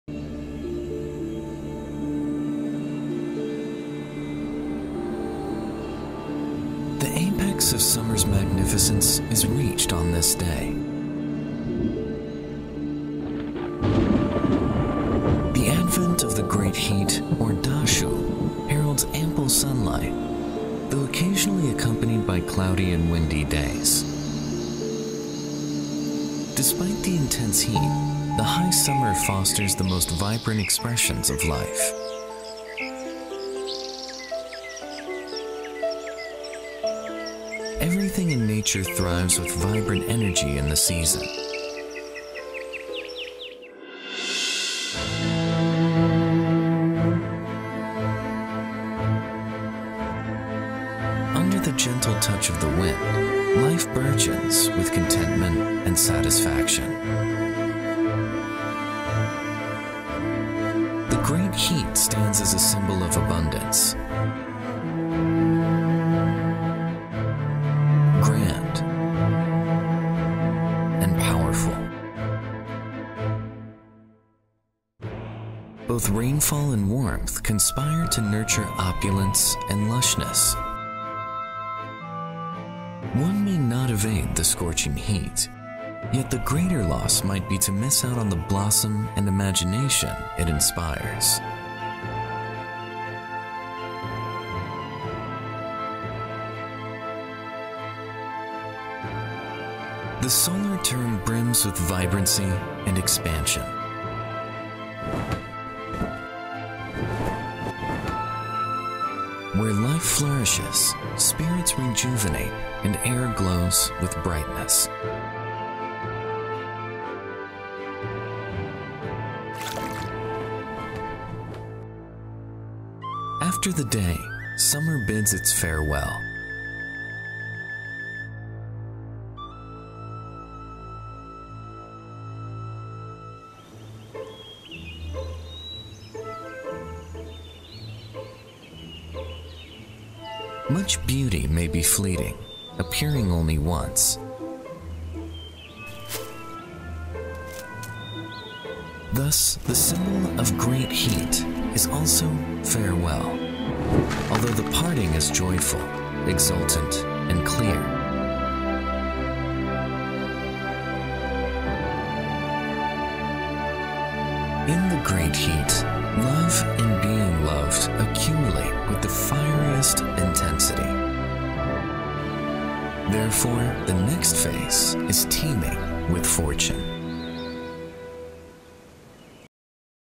Seasons Through Birds' Eyes | Dashu
The apex of summer's magnificence is reached on this day.

The advent of the Great Heat, or Dashu, heralds ample sunlight, though occasionally accompanied by cloudy and windy days. Despite the intense heat, the high summer fosters the most vibrant expressions of life.

Everything in nature thrives with vibrant energy in the season.

Under the gentle touch of the wind, life burgeons with contentment and satisfaction.

The Great Heat stands as a symbol of abundance, grand and powerful.

Both rainfall and warmth conspire to nurture opulence and lushness.

One may not evade the scorching heat. Yet, the greater loss might be to miss out on the blossom and imagination it inspires.

The solar term brims with vibrancy and expansion. Where life flourishes, spirits rejuvenate and air glows with brightness.

After the day, summer bids its farewell. Much beauty may be fleeting, appearing only once.

Thus, the symbol of Great Heat is also farewell, although the parting is joyful, exultant and clear.

In the Great Heat, love and being loved accumulate with the fieriest intensity. Therefore, the next phase is teeming with fortune.